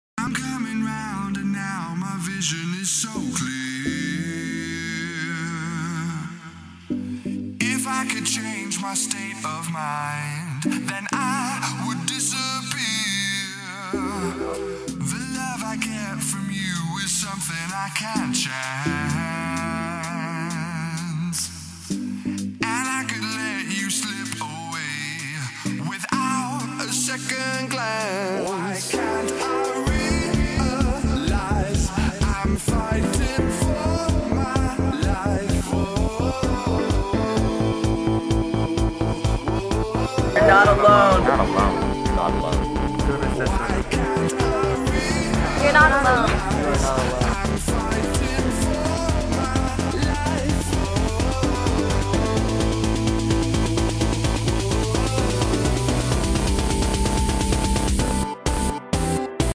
Rise Above
Suicide prevention and stimga video project. enjoy (:
for photography class. and competition
thankyou to all the people who helped by being in the video!